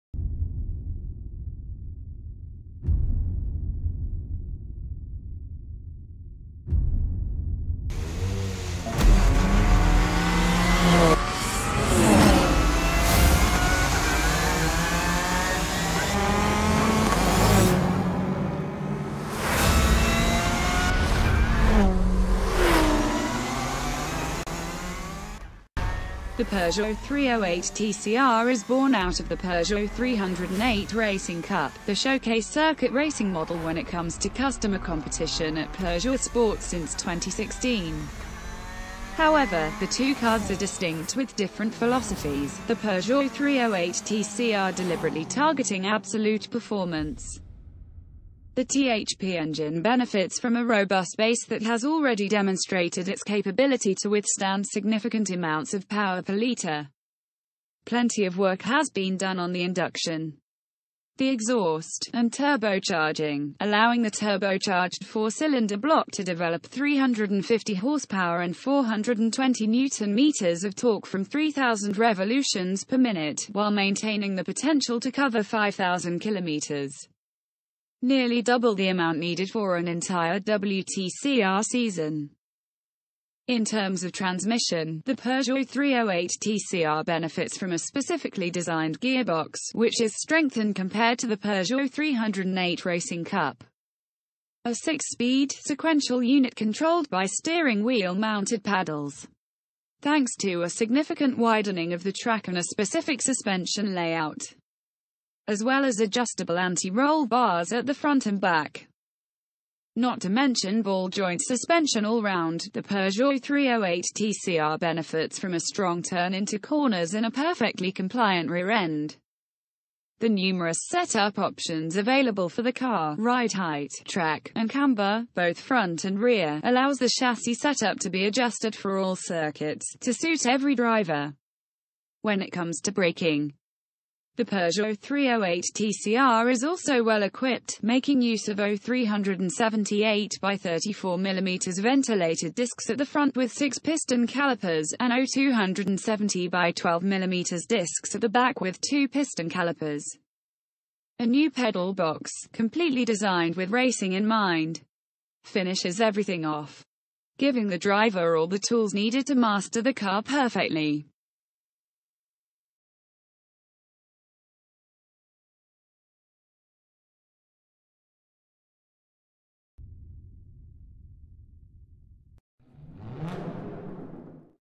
2018 Peugeot 308 TCR Explained
The PEUGEOT 308TCR is born out of the PEUGEOT 308 Racing Cup, the showcase circuit racing model when it comes to customer competition at PEUGEOT SPORT since 2016. However, the two cars are distinct with different philosophies; the PEUGEOT 308TCR deliberately targeting absolute performance.

The THP engine benefits from a robust base that has already demonstrated its capability to withstand significant amounts of power per litre. Plenty of work has been done on the induction, the exhaust, and turbocharging – allowing the turbocharged 4-cylinder block to develop 350 horsepower and 420 Nm of torque from 3000rpm, while maintaining the potential to cover 5000 kilometres: nearly double the amount needed for an entire WTCR season!

In terms of transmission, the PEUGEOT 308TCR benefits from a specifically-designed gearbox (which is strengthened compared to the PEUGEOT 308 Racing Cup): a 6-speed sequential unit controlled by steering wheel-mounted paddles.

Thanks to a significant widening of the track and a specific suspension layout, as well as adjustable anti-roll bars at the front and back, not to mention ball joint suspension all-round, the PEUGEOT 308TCR benefits from a strong turn-in to corners and a perfectly compliant rear end.

The numerous set-up options available for the car (ride height, track, and camber – both front and rear) allows the chassis set-up to be adjusted for all circuits, to suit every driver.

When it comes to braking, the PEUGEOT 308TCR is also well-equipped, making use of Ø 378x34 mm ventilated disks at the front with 6-piston callipers, and Ø 270x12 mm disks at the back with 2-piston callipers.

A new pedal box – completely designed with racing in mind! – finishes everything off, giving the driver all the tools needed to master the car perfectly.

The bodywork of the new PEUGEOT 308TCR has been jointly created by PEUGEOT SPORT and the designers from the ‘CENTRE DE STYLE PEUGEOT’, achieving optimal aerodynamics within an athletic silhouette, thanks especially to plenty of development work using CFD, and an impressive design.

The front bodywork – which has been particularly carefully designed – allows a perfect flow of air towards the radiator and the intercooler, essential for performance and reliability, while the splitter generates maximum possible downforce to provide directional stability. At the back, the rear bodywork is characterised by a large diffuser, to produce more downforce.

The aesthetics of the car are completed by specific 18-inch wheel rims, equipped with tyres in 27-65/18 size.

The order book for the PEUGEOT 308TCR is now open, with a price fixed at 109 000€ (before taxes). A catalogue of options is also available with immediate effect. All the cars will be delivered fully assembled, with the first customers taking delivery from April 2018. Running costs were a key consideration when developing the car, and will come in at around 4.50€ per kilometre (excluding tax and consumables)

Its technical characteristics mean that the PEUGEOT 308TCR is perfectly suited to take on its strongest rivals and delight all drivers looking for the best performance possible, while running costs will please teams.